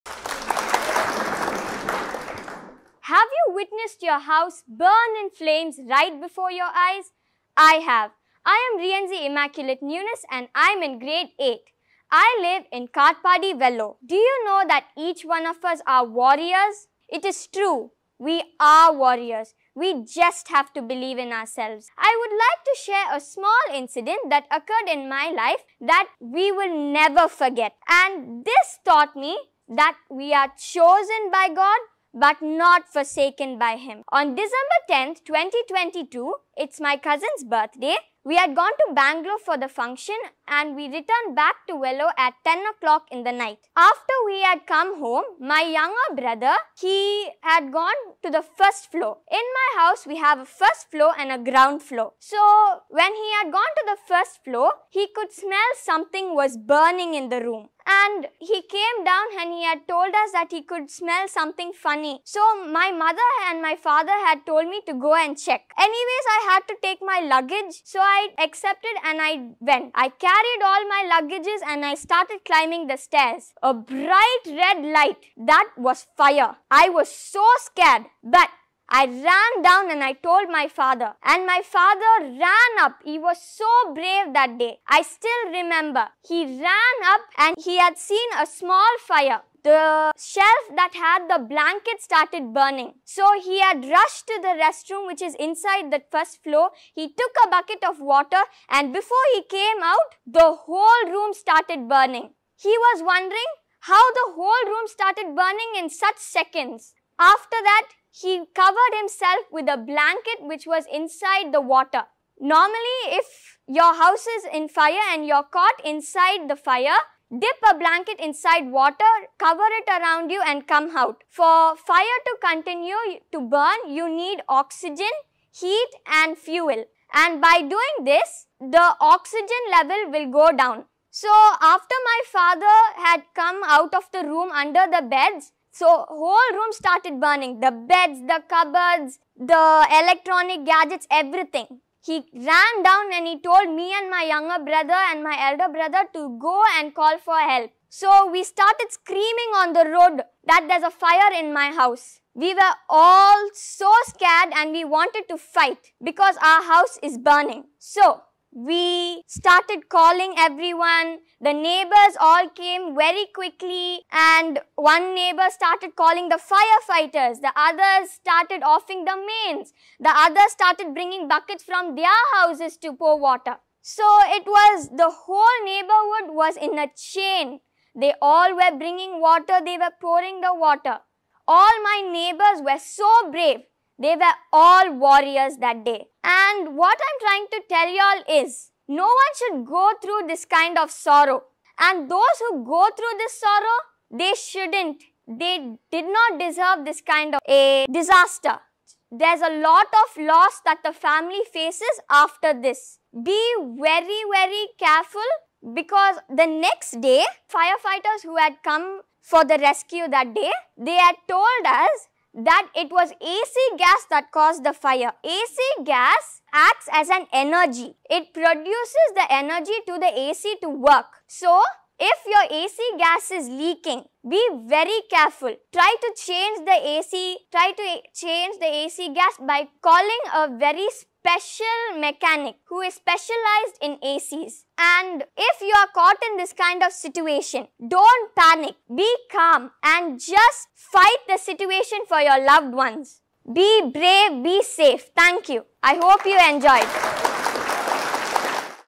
Chosen not Forsaken | TED-Ed Student Talks | Rienzie |story influence academy John Pradeep JL
Prepare to be captivated by the eloquent words of Rienzie Immaculate Nunes, a young speaker, and student of Mayflower School Vellore.

Rienzie shares her empowering message on the topic "Chosen, Not Forsaken" at Alliance Franchise on May 29th.

As a proud participant of the Young Speaker Program by Growth School for Kids, Rienzie embodies the essence of confidence and resilience that we strive to cultivate in our young leaders.

@bingeclass and @growthschoolforkids take immense pride in presenting Rienzie Nunes as our exceptional young speaker.

Her thought-provoking insights are sure to leave a lasting impact, reminding us all of the power we hold within.

Let's empower them to become confident communicators and leaders of tomorrow.

Let's come together and celebrate the unique voices that shape our world. 🌍